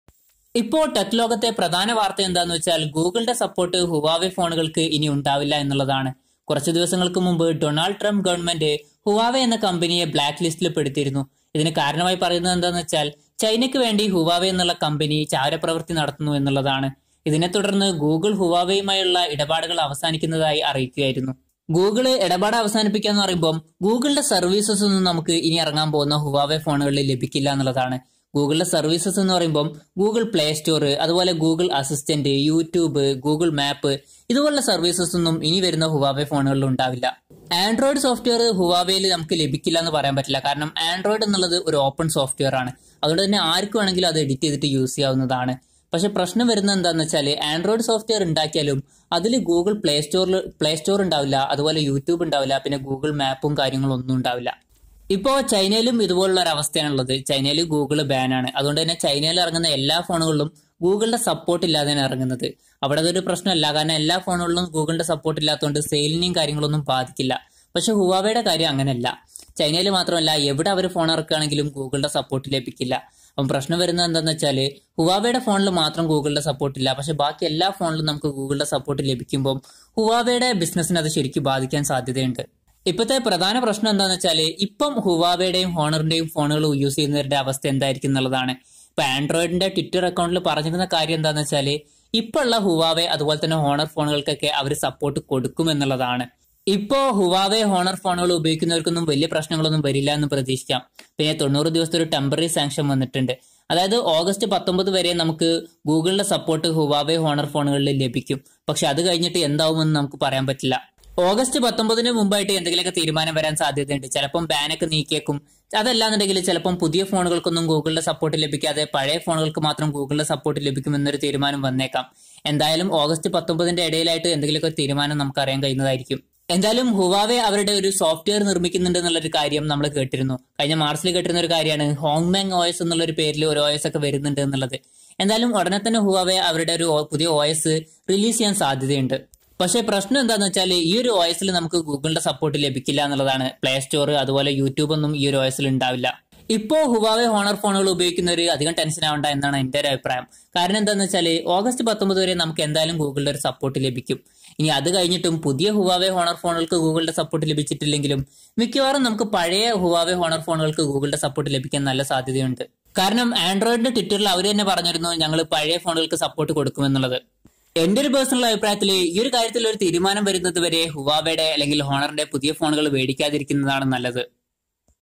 Google Bans Huawei ! Explained In Malayalam | Will It Affect Existing Huawei Or Honor Users ?
Google has banned huawei. Will this decision effect existing huawei or honor users ?